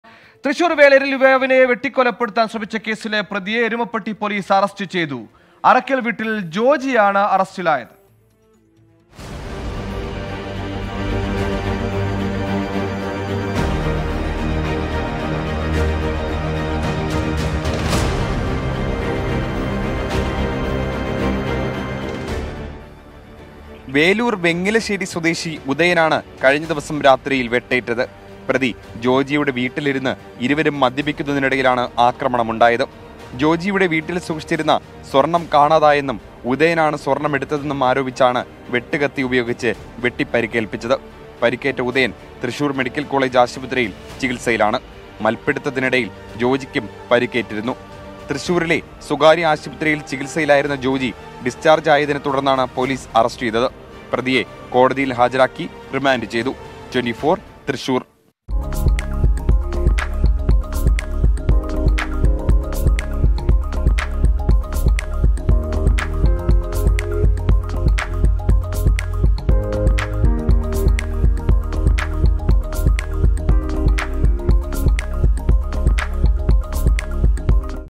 സ്വര്‍ണം മോഷ്ടിച്ചെന്ന ആരോപണത്തിന്റെ പേരില്‍ യുവാവിനെ വെട്ടിക്കൊലപ്പെടുത്താന്‍ ശ്രമം
Thrissur, Murder attempt, Kerala police